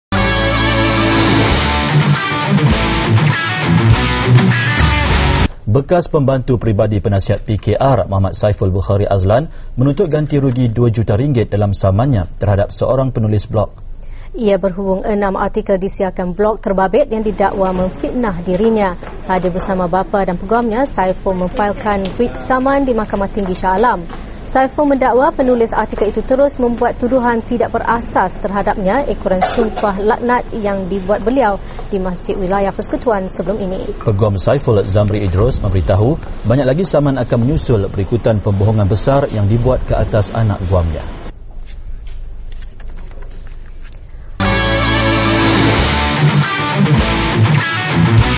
Gantirugi 2 Juta - Saiful
Saiful Bukhari Azlan menuntut gantirugi RM2 Juta daripada penulis blog pahangdaily yang cuba memfitnahnya